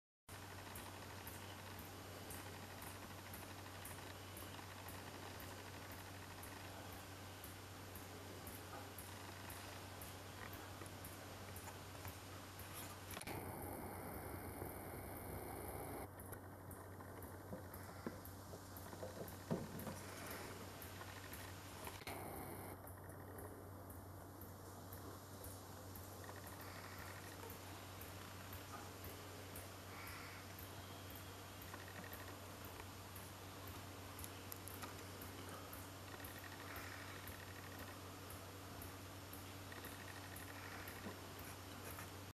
Badger and Cat
Badger comes around every night for apples, the stray cat doesn't like it.